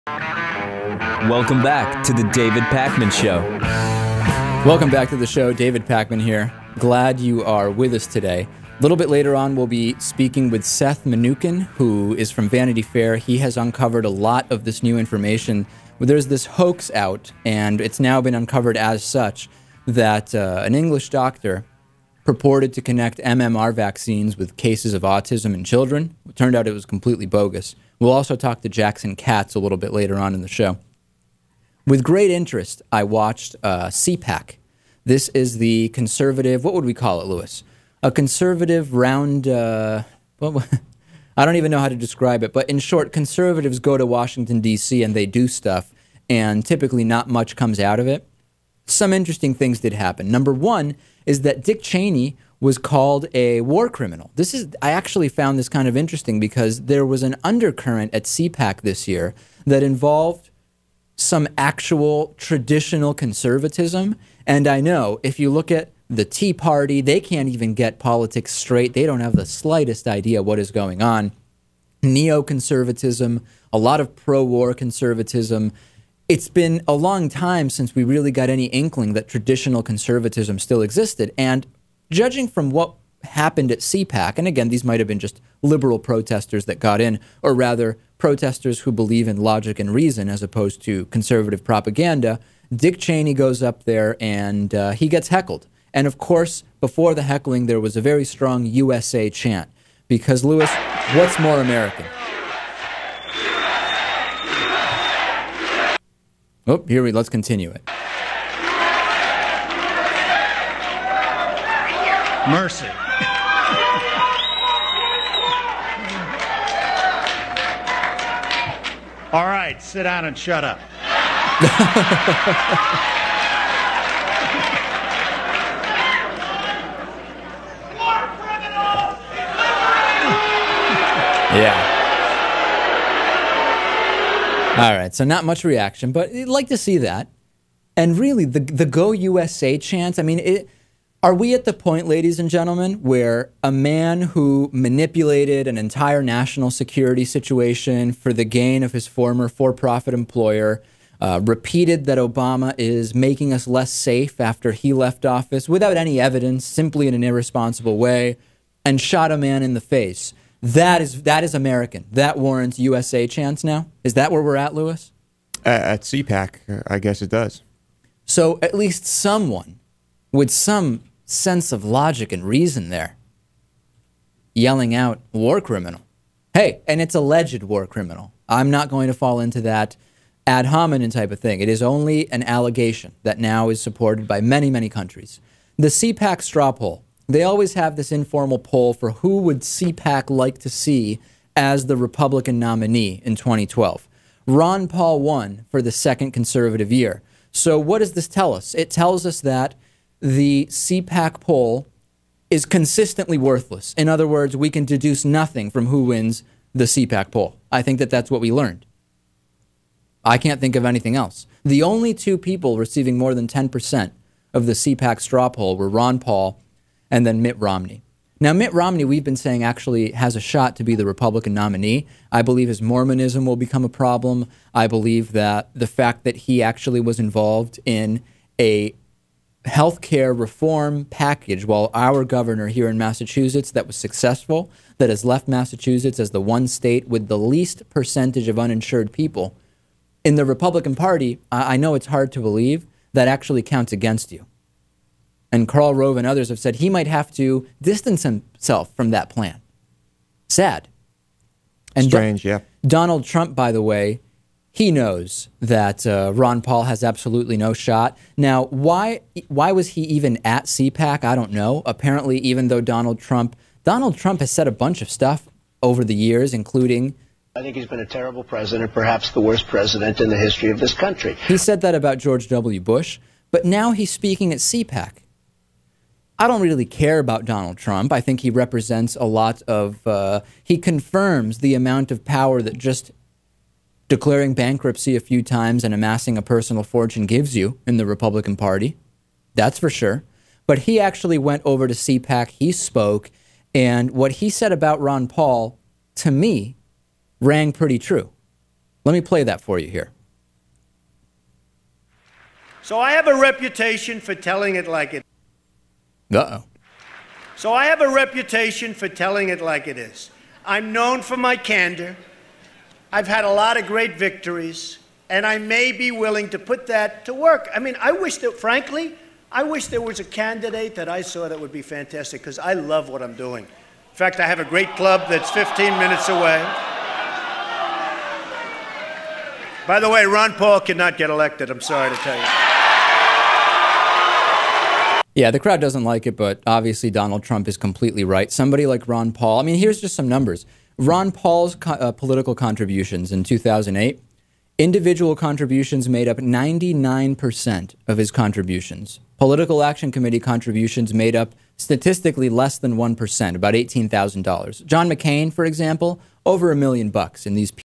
CPAC, Dick Cheney Heckled, Ron Paul Wins Poll but Can't Win, Donald Trump
--CPAC happened in Washington DC over the weekend, including Dick Cheney being called a war criminal, and a CPAC poll won by Ron Paul, confirming how worthless that straw poll is.

--Donald Trump shows up at CPAC and says Ron Paul is unelectable

--On the Bonus Show One third of Russians believe Sun orbits around Earth, talking geothermal energy, a controversial new internet domain extension, much more.

24/7 Voicemail Line & Studio Number: (219)-2DAVIDP